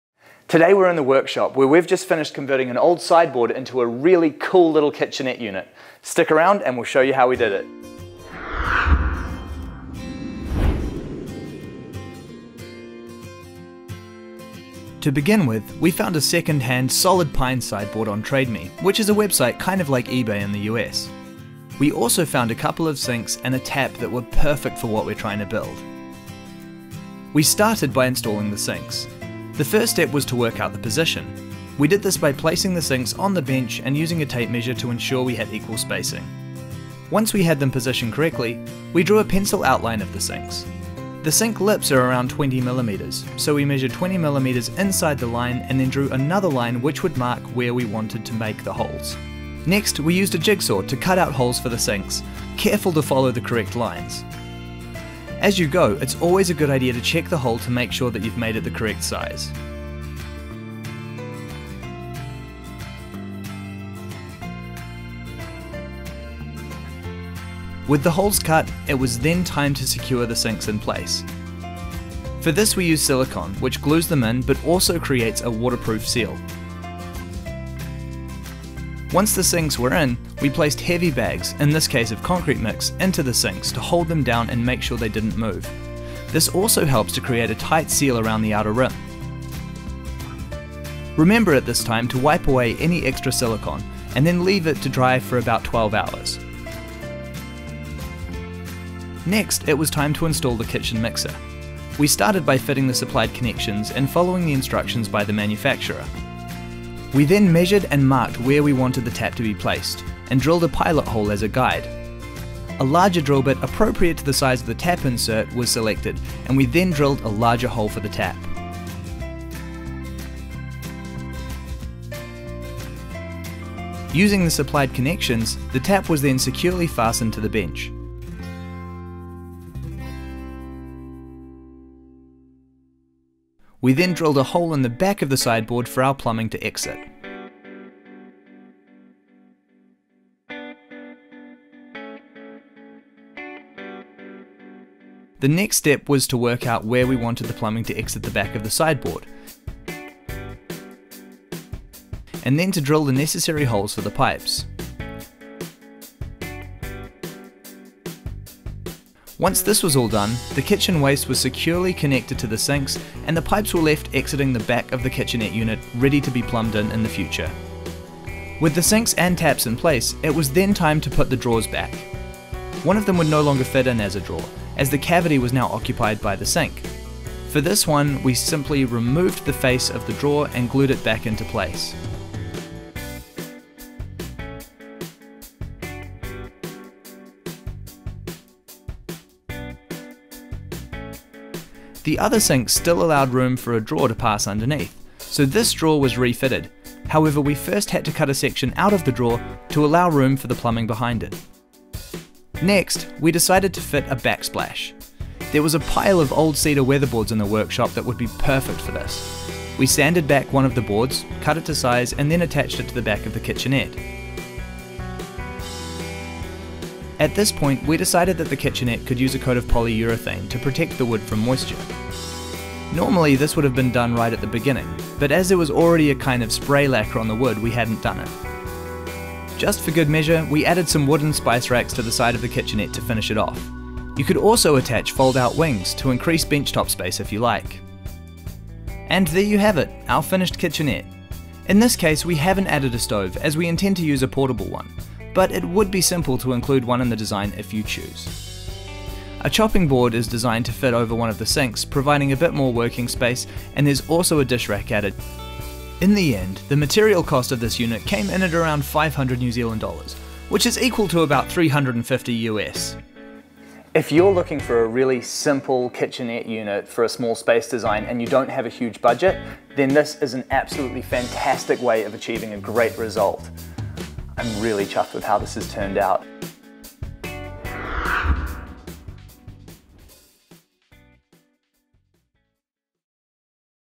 Super-Simple DIY Kitchenette For Small Spaces
This super simple, DIY kitchenette was actually converted from an old sideboard, making the build incredibly simple and very cost effective.

If you're looking to build a kitchenette for a small space such as a tiny house, a motorhome, caravan or sleep-out, then this is a fantastic option.

In our case, we are constructing this kitchenette to be installed in our Lotus Belle tent, which we will be living in while we complete the Tiny House build.

For more information on this (and more small space design ideas), please visit our